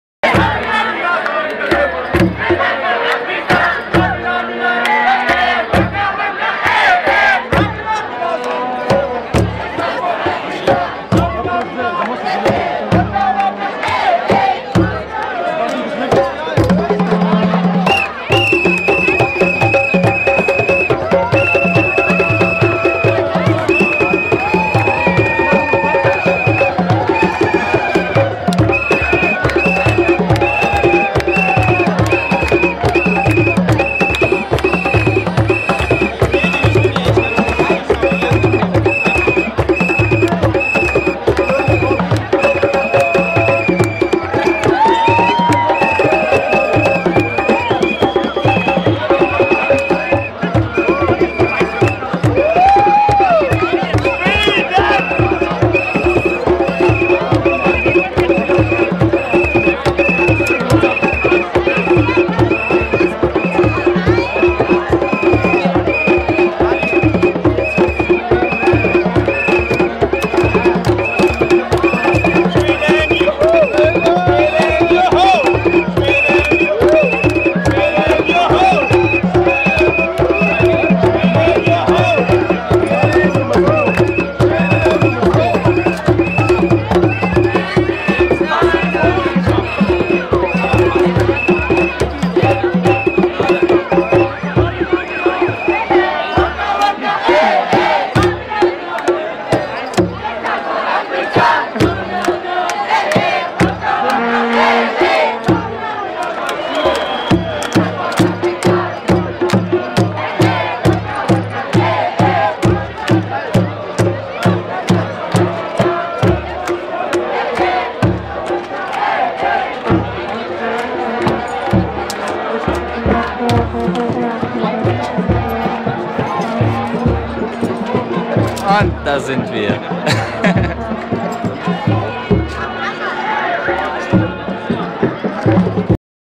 WM 2010 - Feier nach Sieg gegen Argentinien
Afrikanische Trommler machen an der Neckarbrücke in Tübingen ordentlich Stimmung anlässlich des Sieges der Deutschen gegen Argentinien (0:4)!